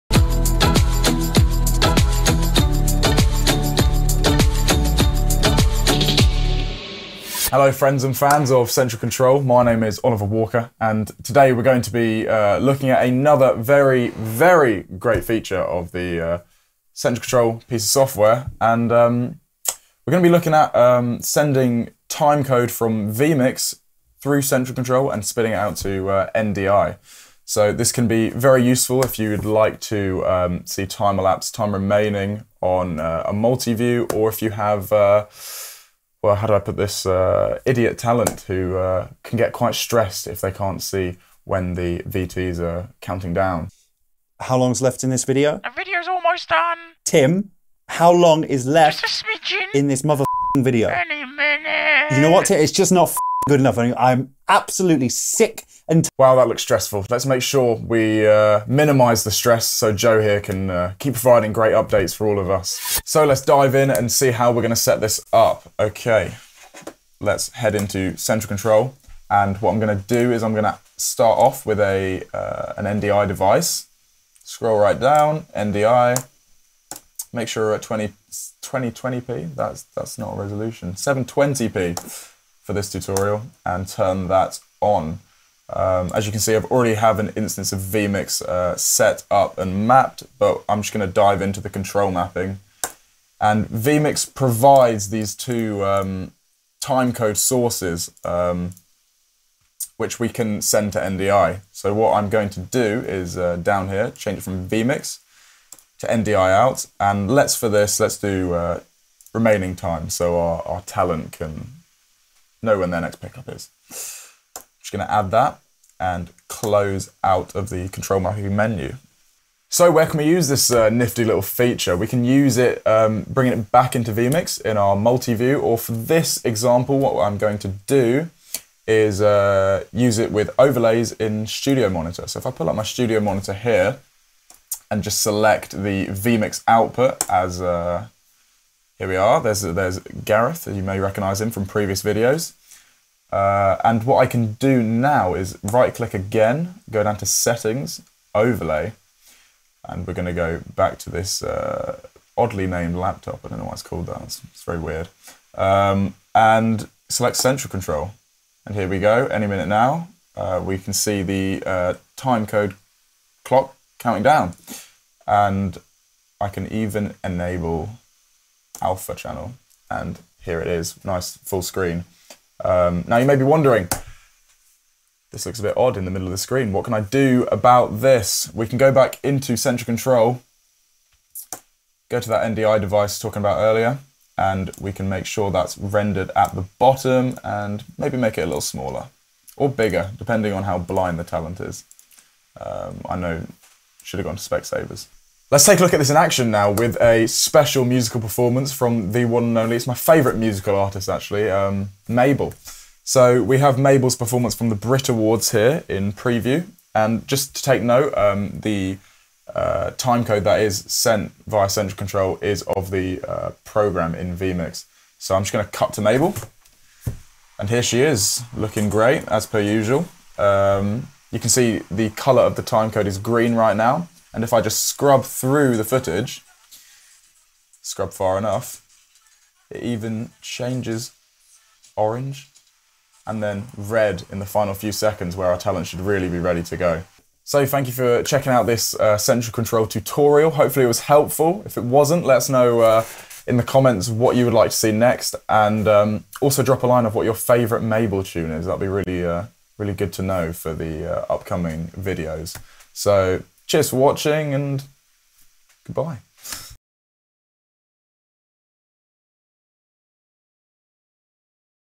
Send vMix Video Playback Timecode via NDI
In this video, we learn how to send video timecode from vMix to an NDI video source. Ideal for use in multiviews or to show to your talent using Studio Monitor's overlay functionality!